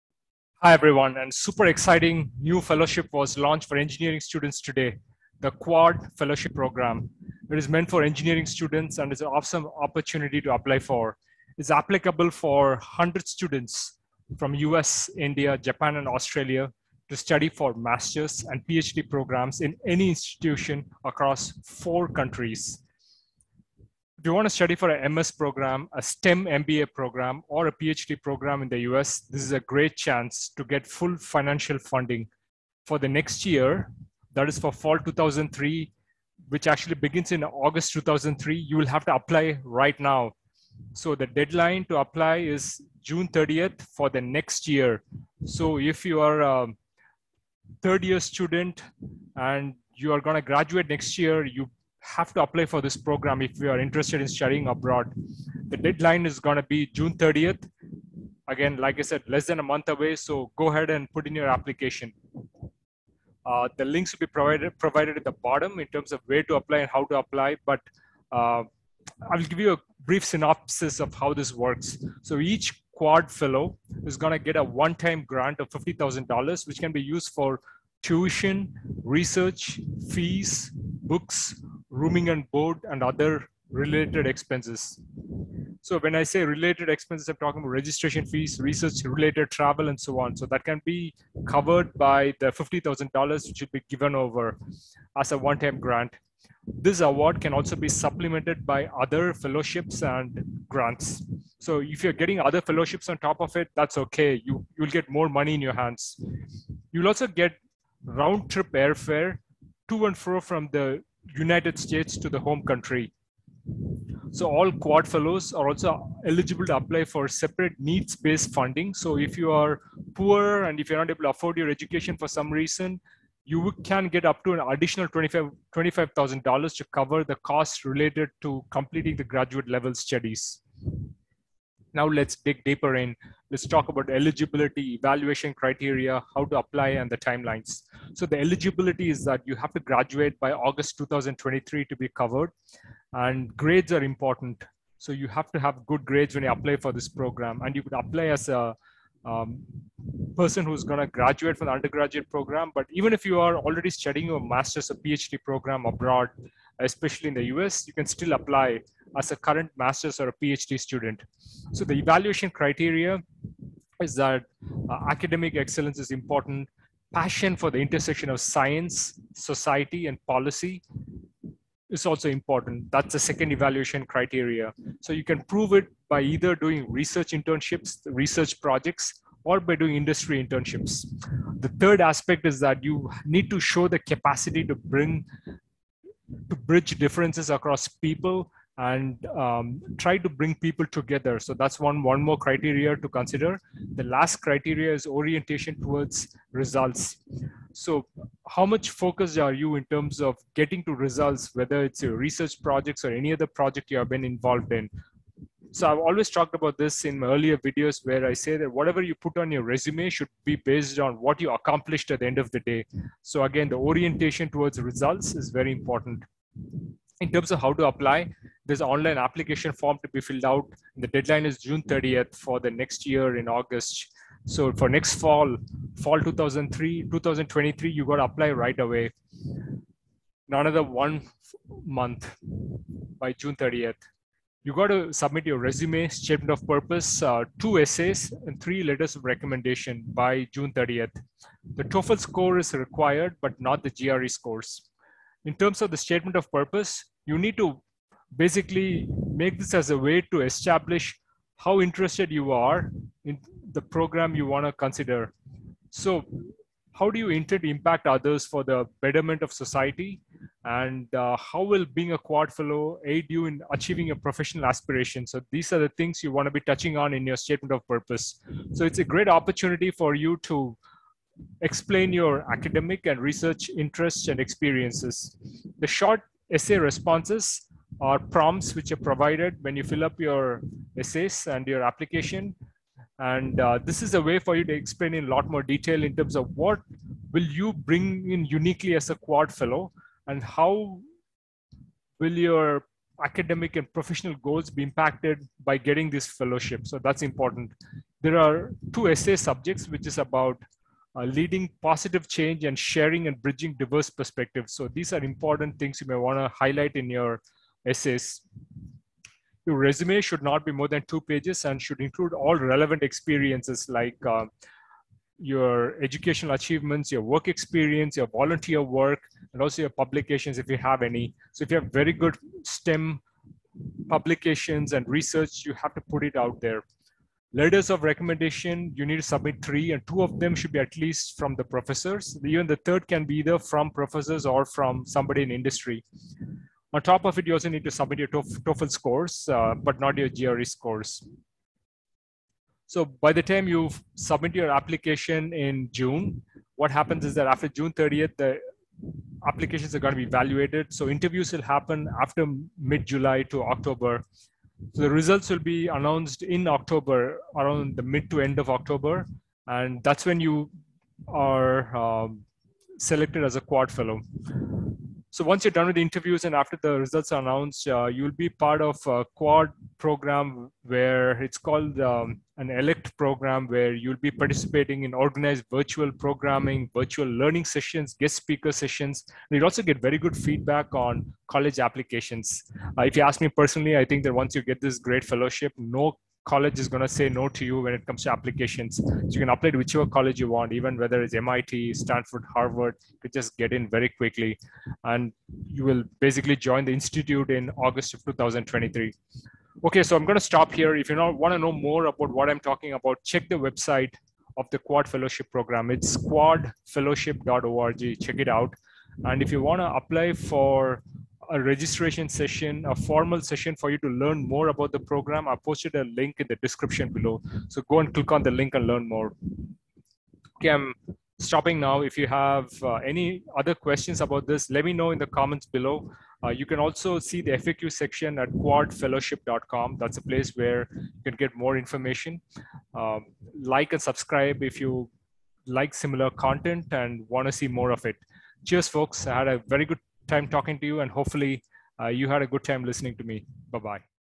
QUAD fellowship announced today I full scholarship for MS, STEM MBA and PhD programs I Apply now
An exciting new fellowship for engineering students was launched today - the Quad fellowship program. It is applicable for 100 students from US, India, Japan and Australia to study for Masters and PhD programs in any institution across the 4 countries. If you wanted to study for a MS, STEM MBA or Ph.D. program in the US, this is a golden chance. For next year, Fall 2023 which refers to August 2023, you have to apply right now. The deadline is June 30, less than a month away.

Apply for the registration session at the link provided in the description